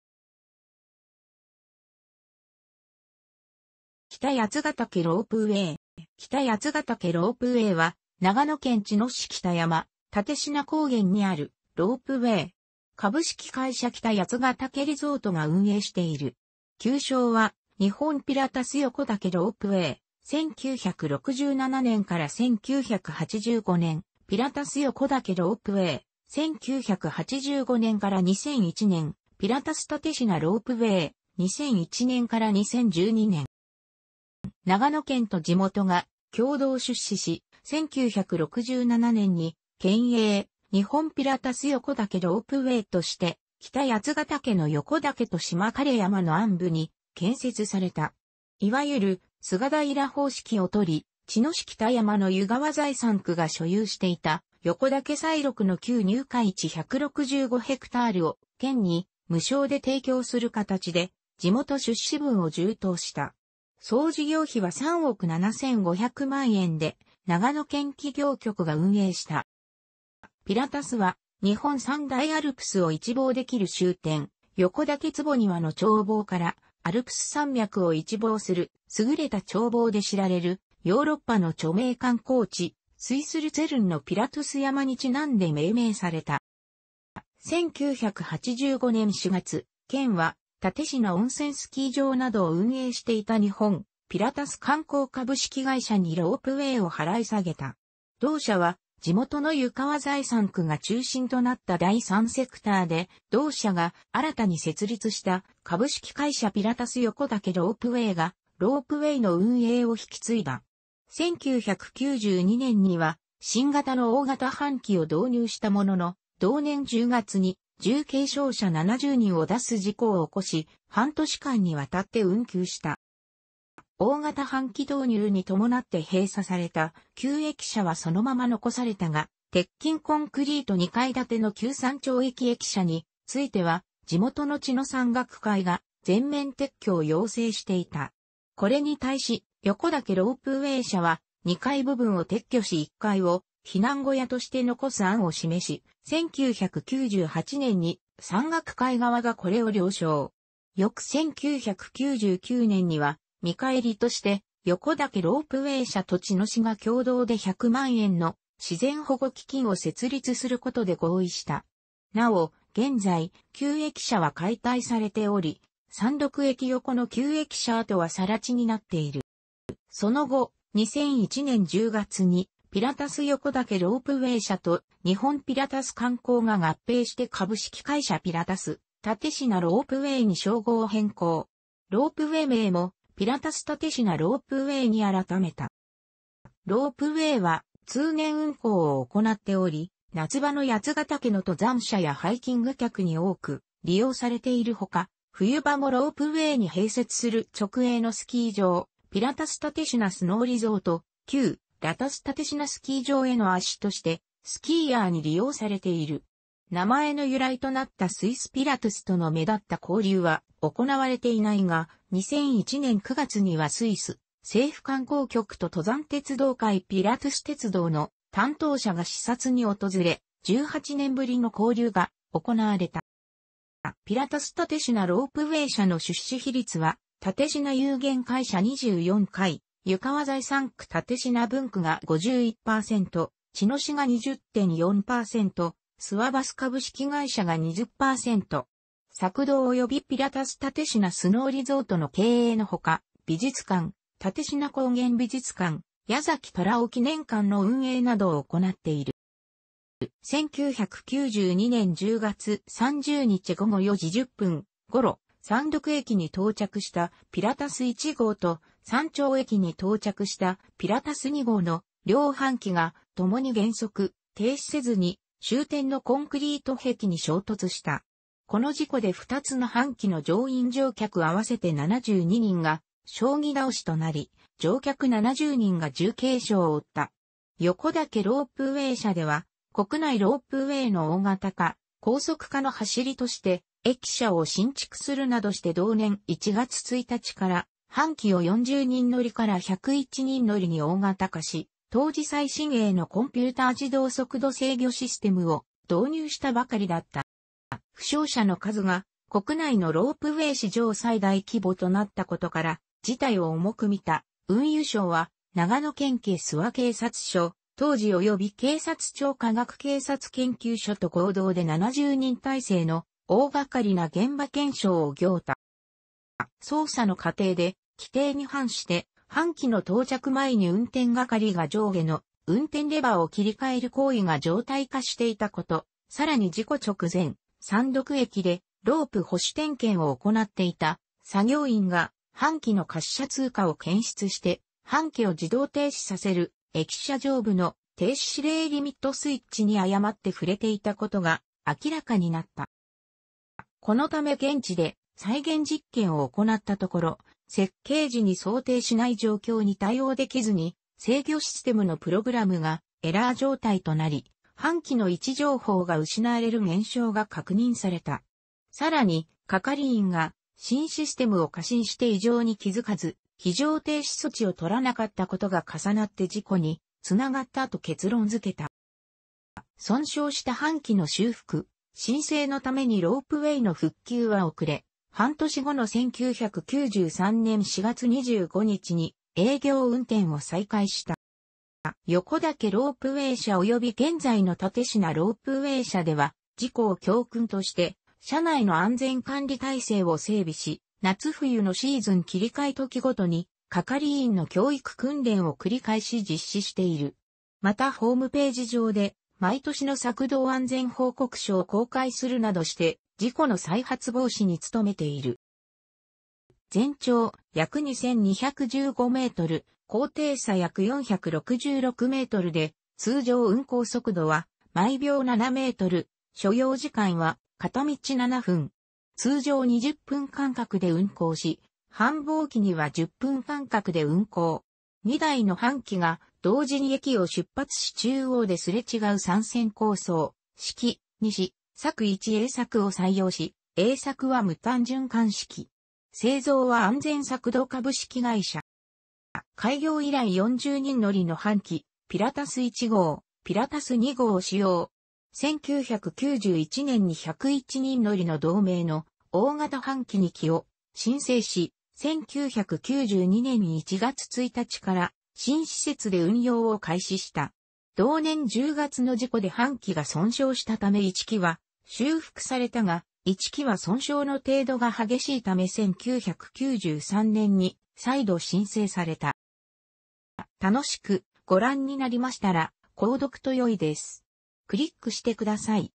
北八ヶ岳ロープウエイ
北八ヶ岳ロープウエイ（きたやつがたけロープウエイ）は、長野県茅野市北山（蓼科高原）にあるロープウェイ。株式会社北八ヶ岳リゾートが運営している。旧称は「日本ピラタス横岳ロープウェイ」（1967年－1985年）、「ピラタス横岳ロープウェイ」（1985年－2001年）、「ピラタス蓼科ロープウェイ」（2001年-2012年）。

長野県と地元が共同出資し、1967年に県営「日本ピラタス横岳ロープウェイ」として、北八ヶ岳の横岳と縞枯山の鞍部に建設された。いわゆる「菅平方式」を取り、茅野市北山の湯川（ゆがわ）財産区が所有していた横岳西麓の旧入会地165ヘクタールを県に無償で提供する形で地元出資分を充当した。総事業費は3億7500万円で、長野県企業局が運営した。

「ピラタス」は、日本三大アルプスを一望できる終点・横岳坪庭の眺望から、アルプス山脈を一望する優れた眺望で知られるヨーロッパの著名観光地、スイス・ルツェルンのピラトゥス山にちなんで命名された。

1985年4月、県は蓼科温泉スキー場などを運営していた日本ピラタス観光株式会社にロープウェイを払い下げた。同社は地元の湯川財産区が中心となった第3セクターで、同社が新たに設立した「株式会社ピラタス横岳ロープウェイ」がロープウェイの運営を引き継いだ。1992年には新型の大型搬器を導入したものの、同年10月に重軽傷者70人を出す事故を起こし、半年間にわたって運休した。

大型搬器導入に伴って閉鎖された旧駅舎はそのまま残されたが、鉄筋コンクリート2階建ての旧山頂駅駅舎については、地元の茅野山岳会が全面撤去を要請していた。これに対し横岳ロープウェイ社は、2階部分を撤去し1階を避難小屋として残す案をしめし、1998年に山岳会側がこれを了承。翌1999年には見返りとして横岳ロープウェイ社と茅野市が共同で1000万円の自然保護基金を設立することで合意した。なお、現在、旧駅舎は解体されており、(現)山麓駅横の旧駅舎跡は更地になっている。

その後、2001年10月にピラタス横岳ロープウェイ社と日本ピラタス観光が合併して株式会社ピラタス蓼科ロープウェイに商号を変更。ロープウェイ名も「ピラタス蓼科ロープウェイ」に改めた。

ロープウェイは通年運行を行っており、夏場の八ヶ岳の登山者やハイキング客に多く利用されているほか、冬場もロープウェイに併設する直営のスキー場「ピラタス蓼科スノーリゾート」（旧・ピラタス蓼科スキー場）への足としてスキーヤーに利用されている。名前の由来となったスイス・ピラトゥスとの目立った交流は行われていないが、2001年9月にはスイス政府観光局と登山鉄道会社ピラトゥス鉄道の担当者が視察に訪れ、18年ぶりの交流が行われた。

ピラタス蓼科ロープウェイ社の出資比率は蓼科有限会社二四会（湯川財産区蓼科分区）が51%、茅野市が20.4%、諏訪バス株式会社が20%。索道およびピラタス蓼科スノーリゾートの経営のほか、美術館「蓼科高原美術館・矢崎虎夫記念館」の運営などを行っている。

1992年10月30日午後4時10分ごろ、山麓駅に到着した「ピラタス1号」と、山頂駅に到着した「ピラタス2号」の両搬器が、ともに減速・停止せずに終点のコンクリート壁に衝突した。この事故で2つの搬器の乗員乗客合わせて72人が将棋倒しとなり、乗客70人が重軽傷を負った。横岳ロープウェイ社では国内ロープウェイの大型化・高速化のはしりとして、駅舎を新築するなどして同年1月1日から搬器を40人乗りから101人乗りに大型化し、当時最新鋭のコンピューター自動速度制御システムを導入したばかりだった。

負傷者の数が国内のロープウェイ史上最大規模となったことから、事態を重く見た運輸省は長野県警諏訪警察署（当時）および警察庁科学警察研究所と合同で70人体制の大がかりな現場検証を行った。

捜査の過程で、規定に反して搬器の到着前に運転係が上下の運転レバーを切り替える行為が常態化していたこと、さらに事故直前、山麓駅でロープ保守点検を行っていた作業員が、搬器の滑車通過を検出して搬器を自動停止させる駅舎上部の「停止指令リミットスイッチ」に誤って触れていたことが明らかになった。

このため現地で再現実験を行ったところ、設計時に想定しない状況に対応できずに制御システムのプログラムがエラー状態となり、搬器の位置情報が失われる現象が確認された。さらに、係員が新システムを過信して異常に気付かず、非常停止措置を取らなかったことが重なって...